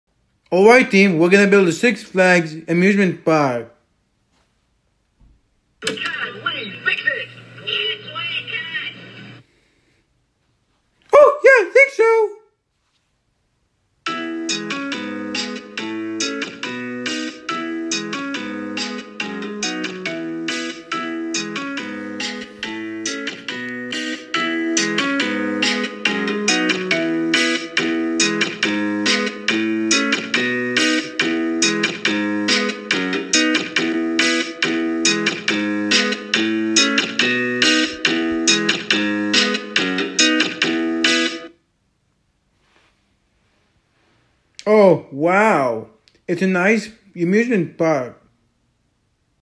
Bob The Builder and the Machines Builds a Six Flags Theme Park
I made the Characters from Bob The Builder Builds a Six Flags Theme Park
Starring
• Bob
• Wendy
• Scoop
• Muck
• Dizzy
• Roley
• Lofty
• Skip
• Jackaroo
• Tumbler